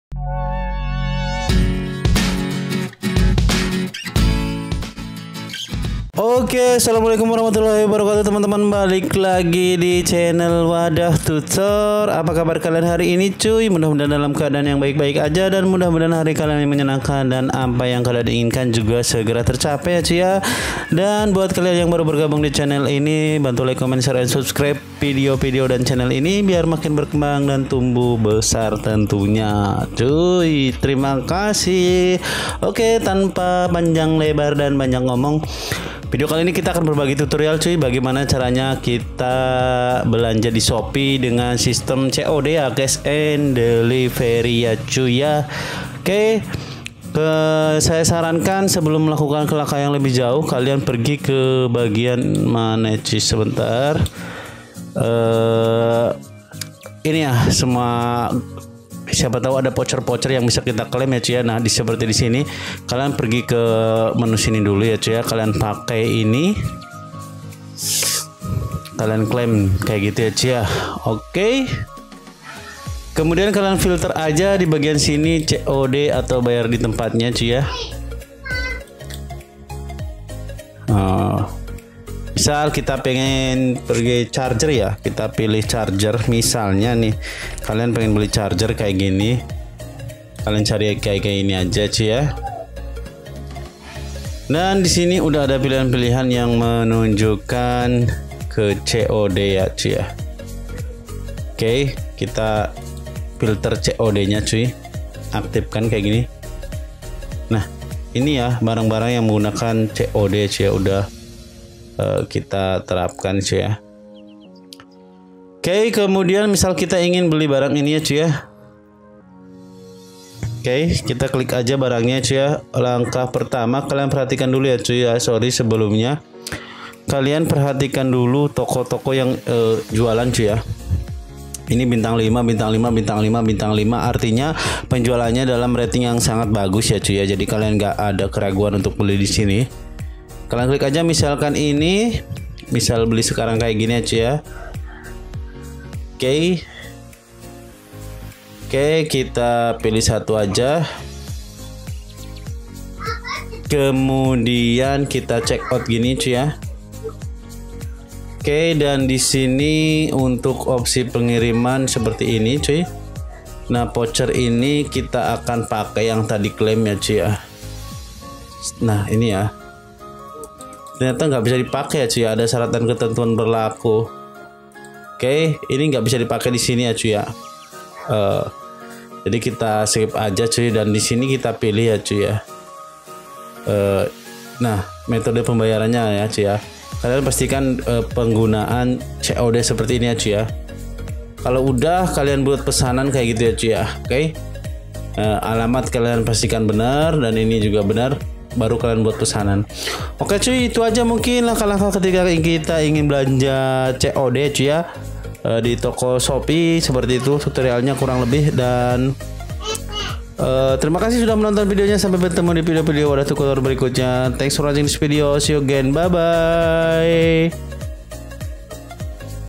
CARA BELANJA COD DI TOKO ONLINE SHOPEE TERBARU 2022
asalamualakium cuy..apa kabar kalian mudah mudahan dalam keadaan yang baik baik aja mudah mudahan apa yang kalian inginkan segera tercapai
dan divideo kali inikita akan berbagi cara bagaimana kalian bisa belanja di shopee dengan cara cod .
semoga vbermanfaat untuk kita semua ya cuy,,,
bantu suppoort channel ini dengan car alike komen share n subsribe video ini

hatur nuhun terimakasih..